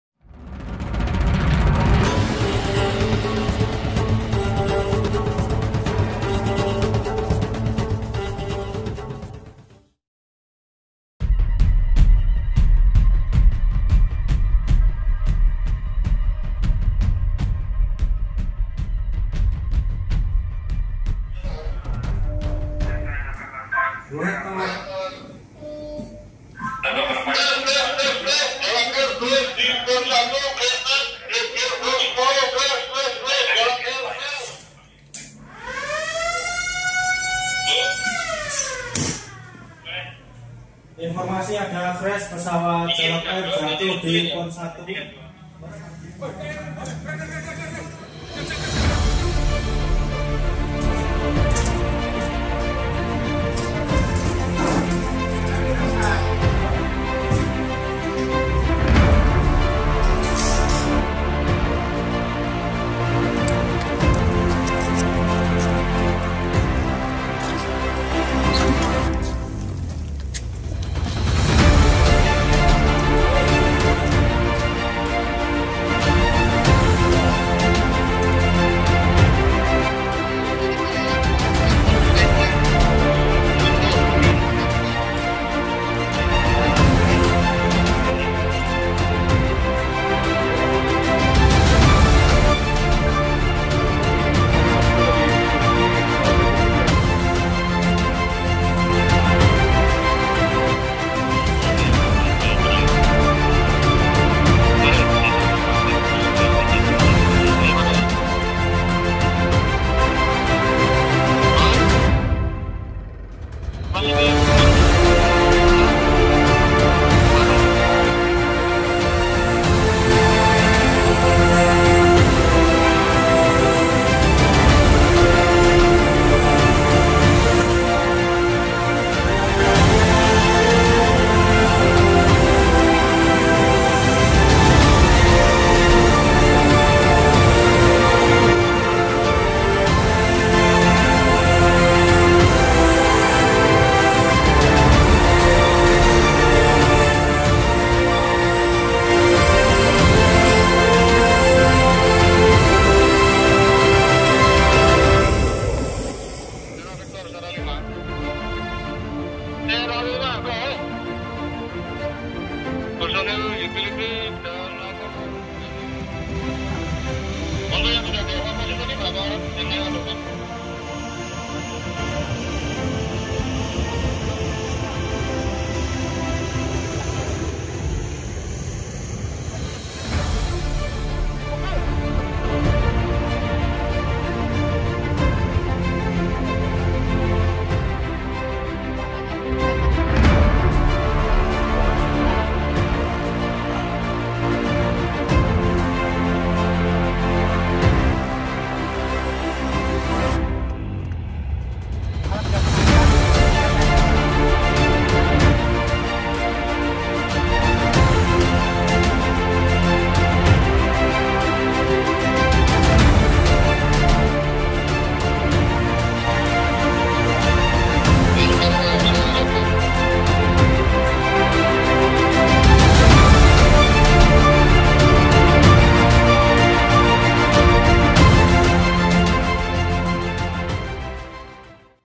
WET DRILL || WATER RESCUE || REGU DELTA || ARFF JUANDA
Latihan Pemadaman dan Pertolongan Kecelakaan Helikopter di Wilayah Perairan Bandar Udara Internasional Juanda

Pada latihan kali ini di skenariokan Helikopter Bell 412 Registrasi PK-JNI Rute SUB-MDN mengalami crash lalu jatuh dan terbakar di Pond 1 Bandar Udara Internasional Juanda

Helikopter PK-JNI terbakar dan para penumpang berusahan menyelamatkan diri di area perairan. ARFF yang menerima informasi segera menuju lokasi dan segera melakukan prosedur pemadaman , dikarenakan berada di area perairan tim ARFF menerjukan Rubber Boat. ditemukan korban 1 orang luka ringan dan 3 orang MD

Latihan ini dilaksanakan oleh Regu Delta